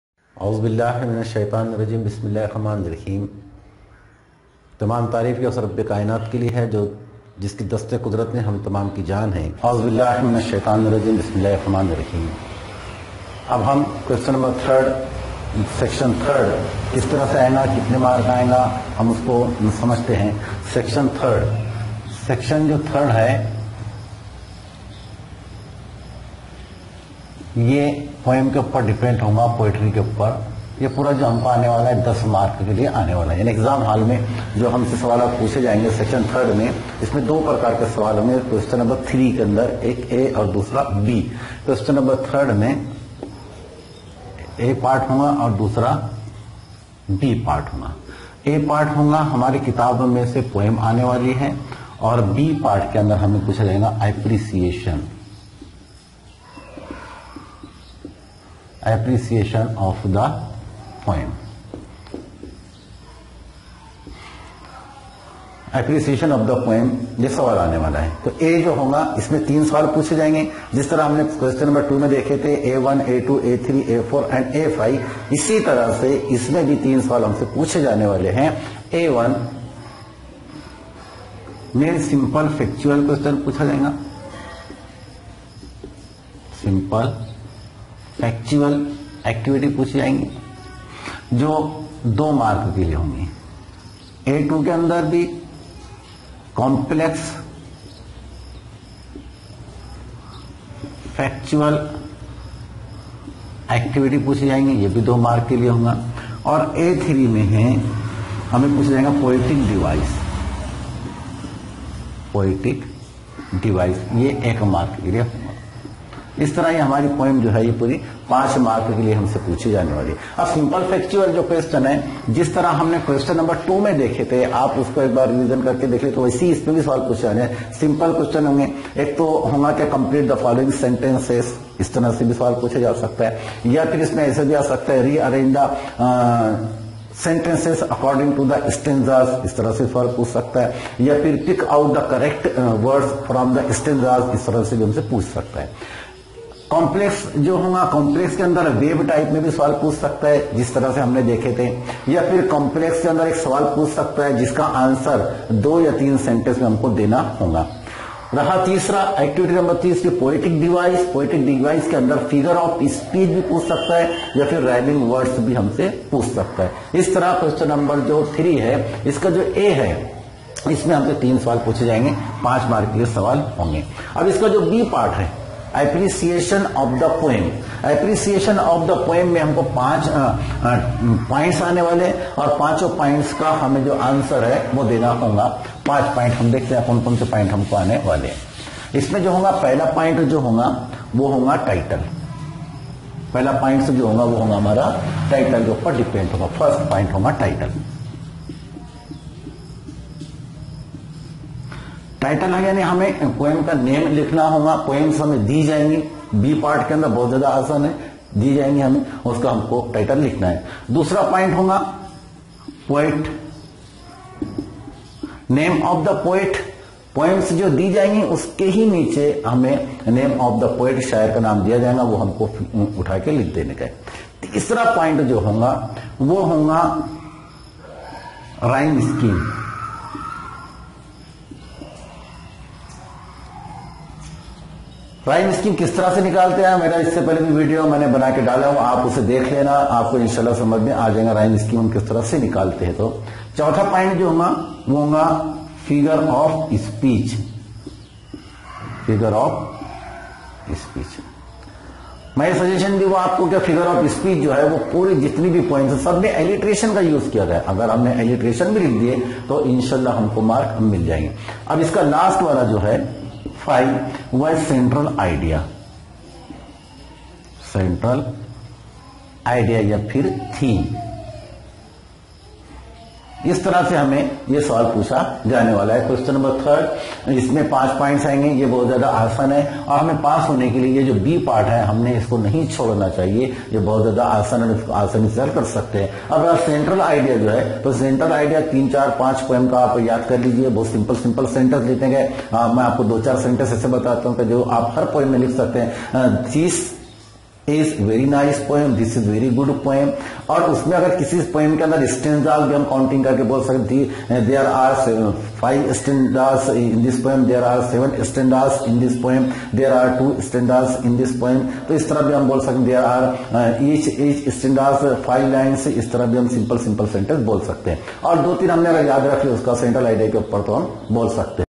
How to solve Q.3 A and B IX and X English by Salim Khan
For Education
We can easily obtain the marks by solving this question and B part is very simple anybody can easily solve it after watching this video.